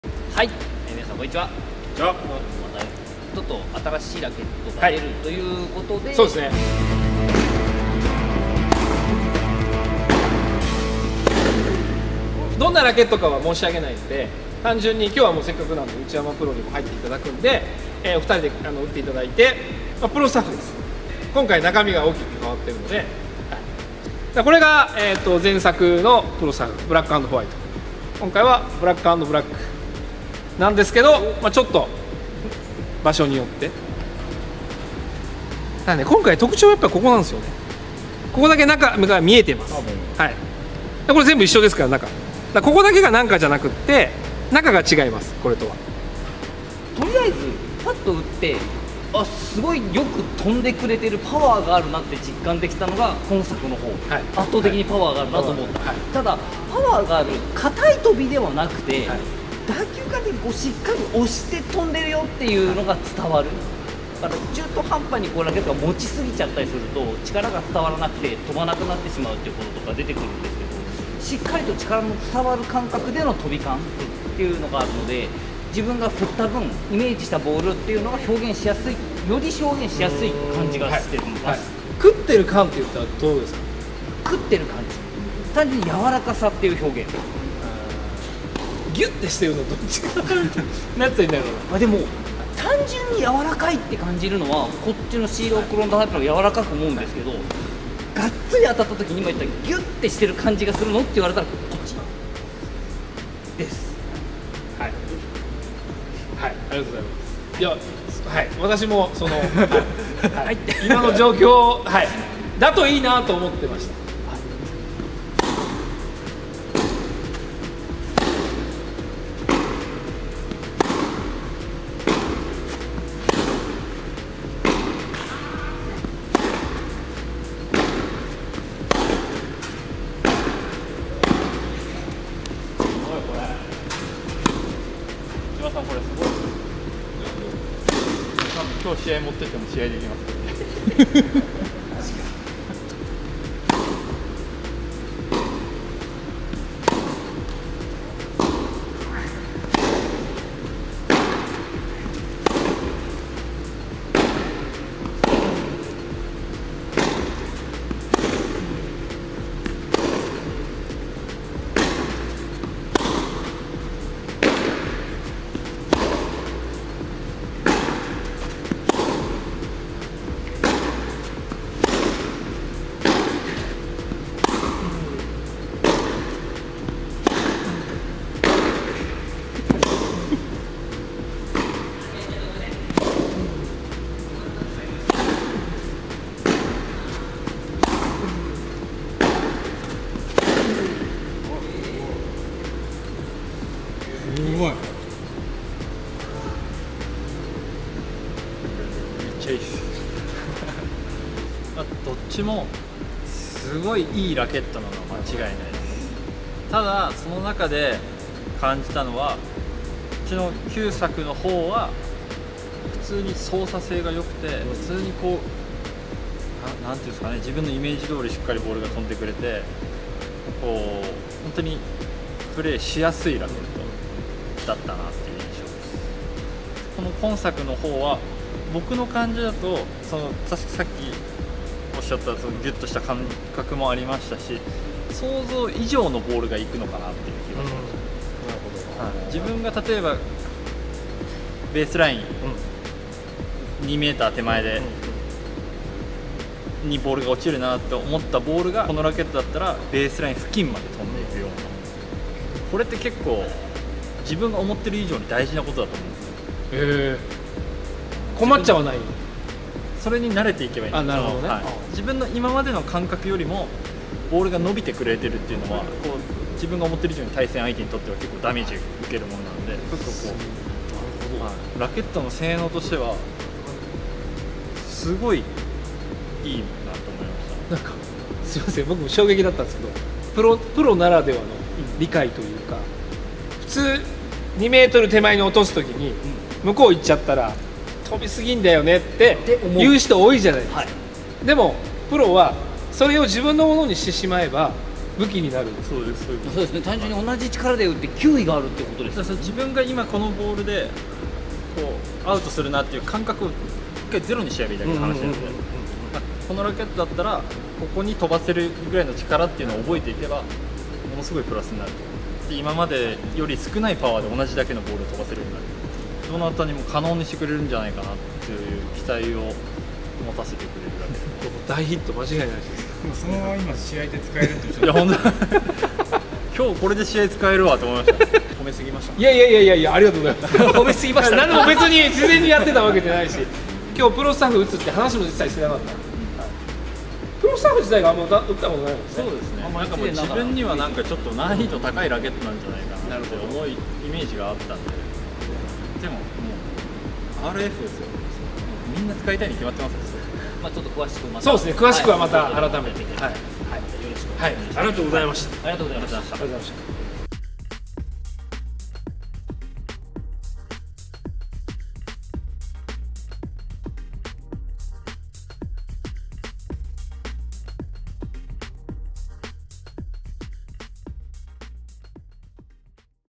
【Fukky'sインプレ】内山靖崇プロ × Wilson PRO STAFF RF97 AUTOGRAPH v13.0
ウイルソン13代目のプロスタッフRF97（フェデラーモデル）を日本代表 内山靖崇プロが初打ち！！13代目の原点進化
【Wilson】PROSTAFF v13.0シリーズ（2020年9月末発売予定）